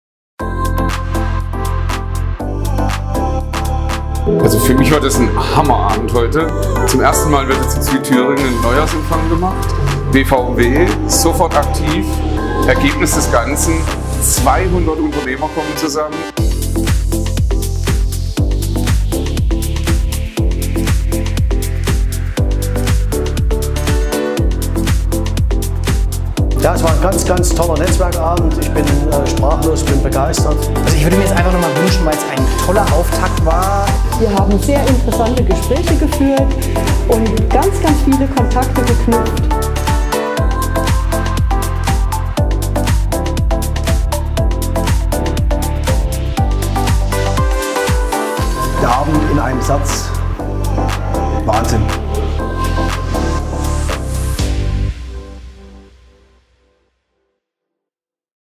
1. Südthüringer Neujahrsempfang in Suhl
Über 200 Unternehmer kamen am 28. Januar zum Neujahrsempfang nach Suhl. Unter dem Motto „Zukunft. Wirtschaft. Erfolgreich.“ wurde der Startschuss für ein erfolgreiches und vernetztes Jahr 2020 gelegt.

Unser großer Dank gilt vor allem unserem Mitveranstalter, der Faszination Media + Event GmbH.